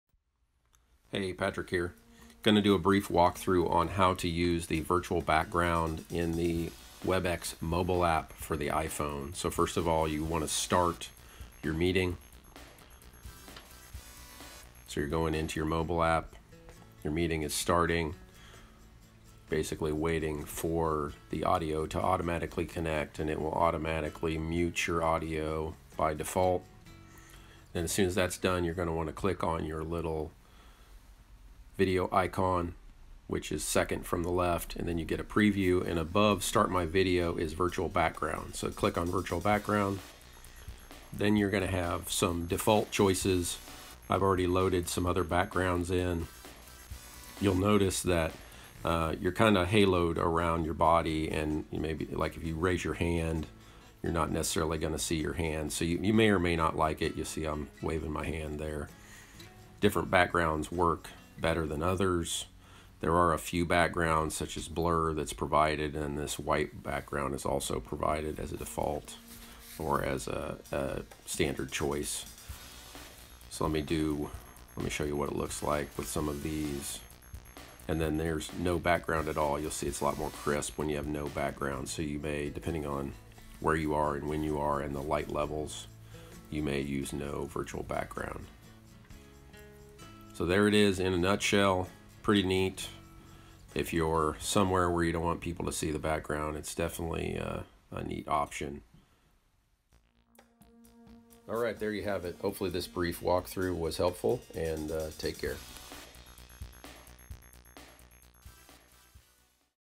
Cisco Webex Meetings Mobile Ap. - iPhone How to - Virtual background feature walk-thru!!
Cisco Webex Meetings Mobile Ap. - iPhone
How to deploy the Virtual background feature exclusively available for the iPhone.
Deploy a background blur or one of a few background samples provided by Webex or use your own.